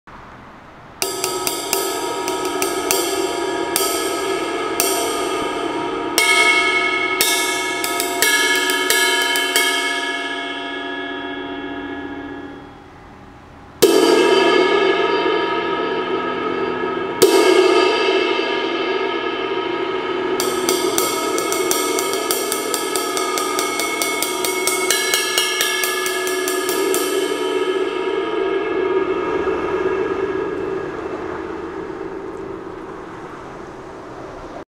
Zildjian 22” K Constantinople Medium Ride Demo
Sound demo of a Zildjian 22” K Constantinople Medium Ride. This ride is currently for sale on Reverb.